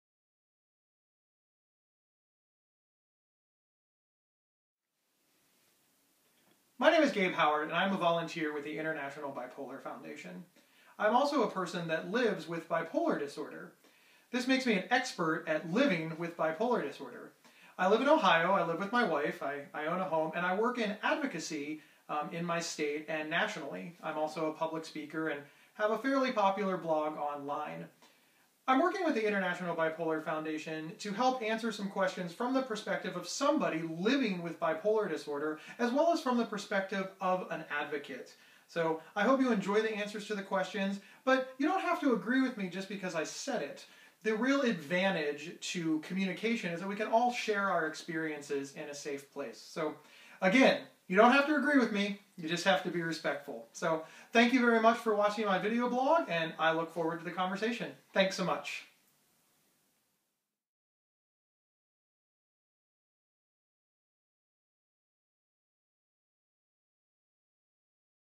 Gabe Howard Introduction
Gabe Howard introduces his video blog series where he will share advice based on his experience living with bipolar disorder.